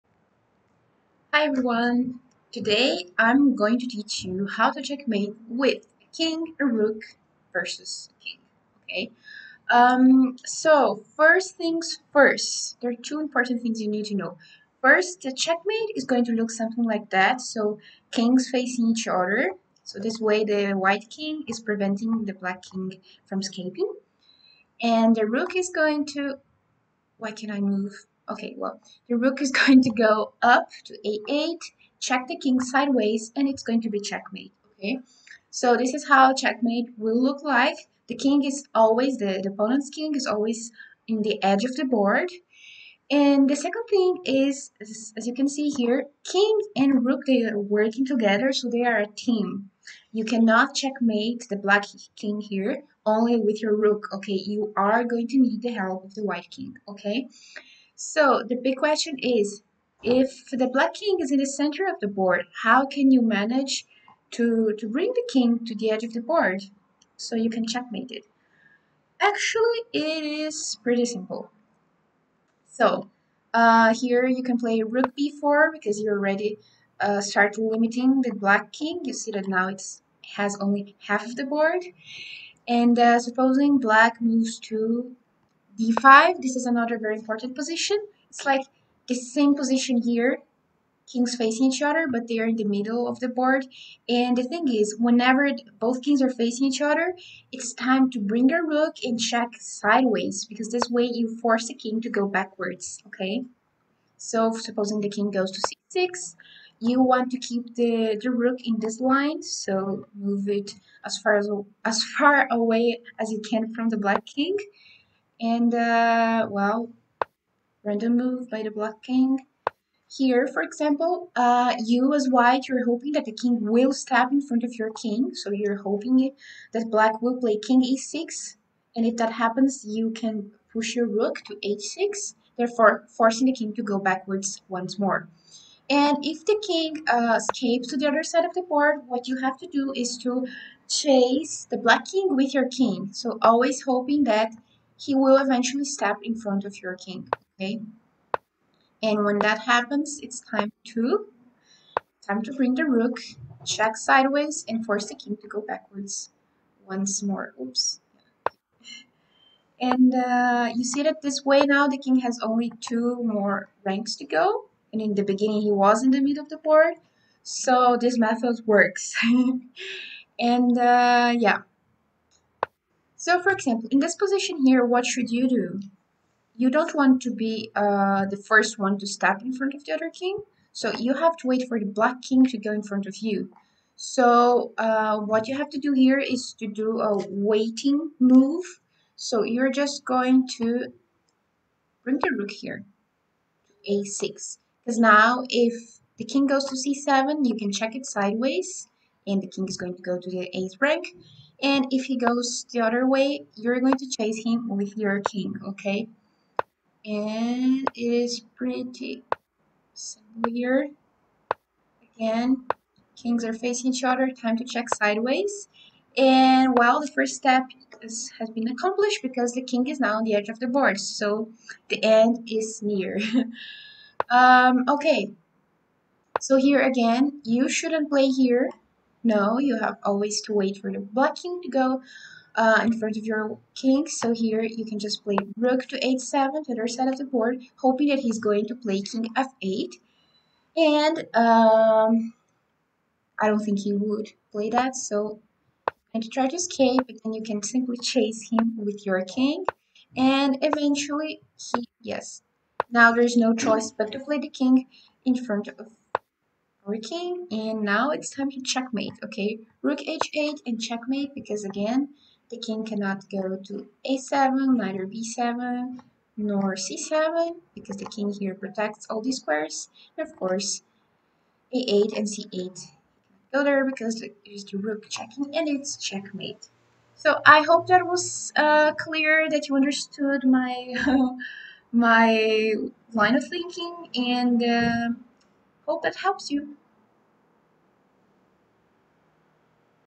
Chess in 5 minutes: how to checkmate with King+Rook (an explanation for kids)
This is certainly not the quickest way to checkmate with king+rook, but I find it to be the easiest way when teaching kids!
Hope it helps someone out there :)